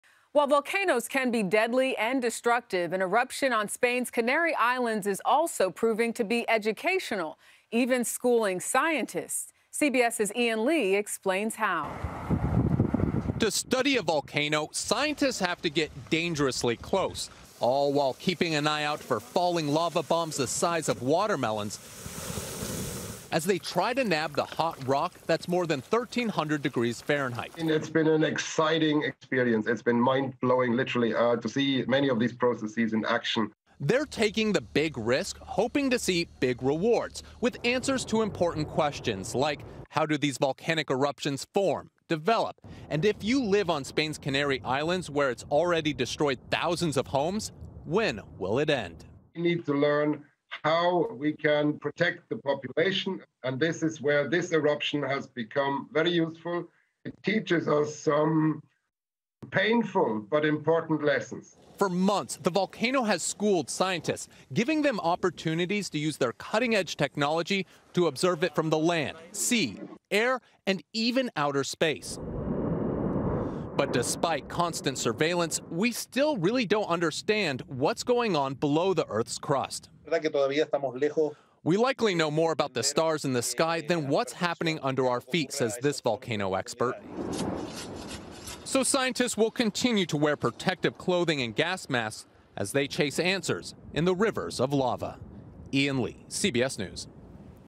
Scientists and students learn from volcanic eruption
The volcano on Spain's La Palma island has provided an opportunity for  scientists and students to get up close with this destructive force of nature. Ian Lee reports on what they're hoping to learn.